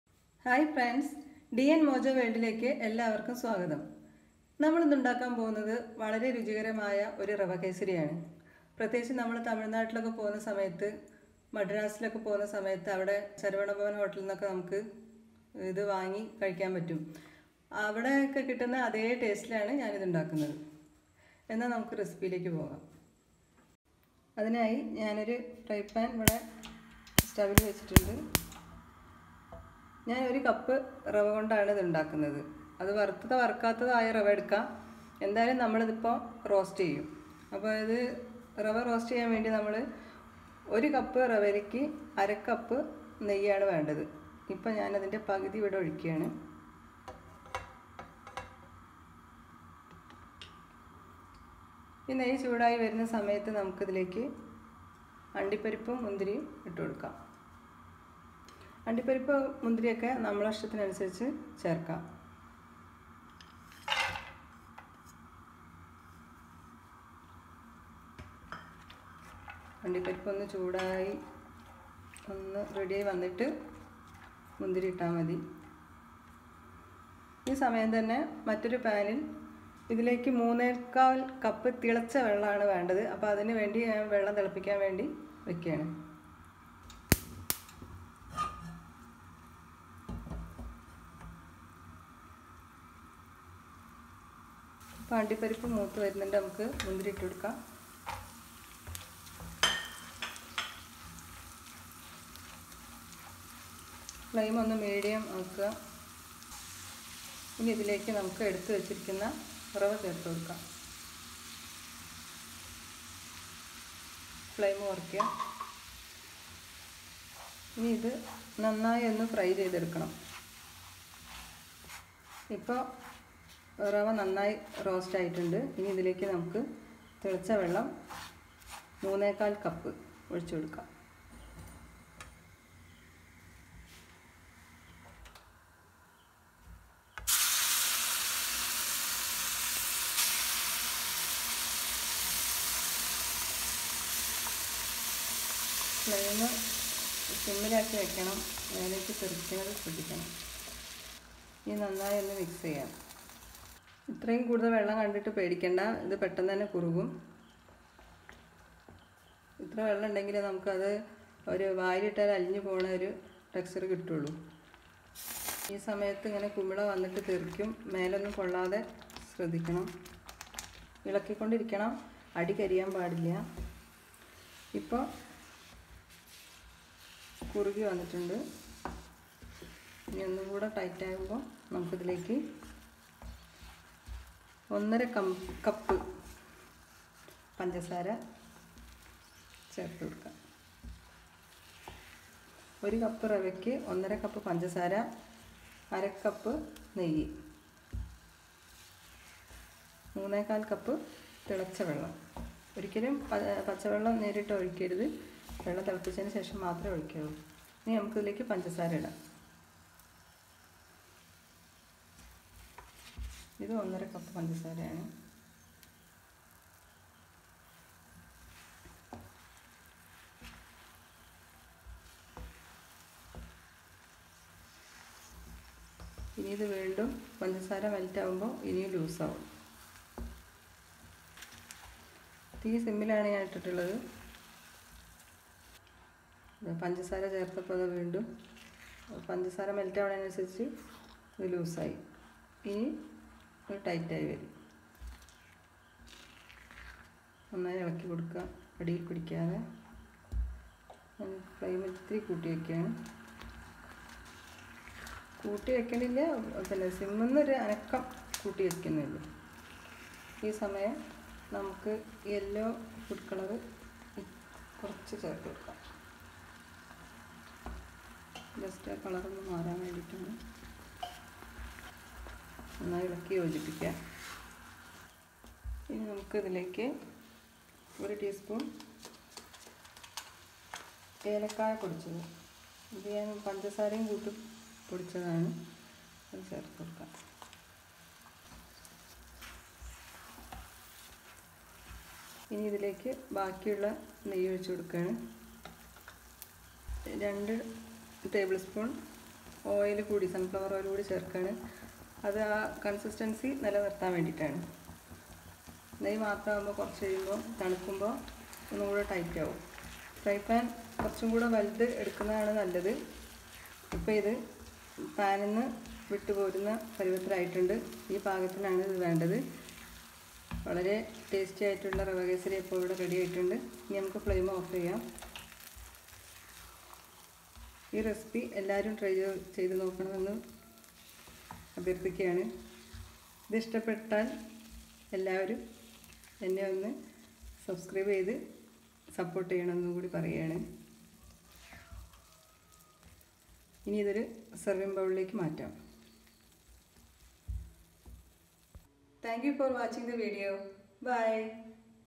How to make delicious rava kesari in malayalam. DN mojo world. Episode: 1
Ingredients:
 Rava / Semolina --1 cup
 Boiled water--3 1/4 cup
 Sunflower oil --2 table spoon
 Cardamom  powder-- 1 tea spoon
 Cashew nuts
 Raisins
 food colour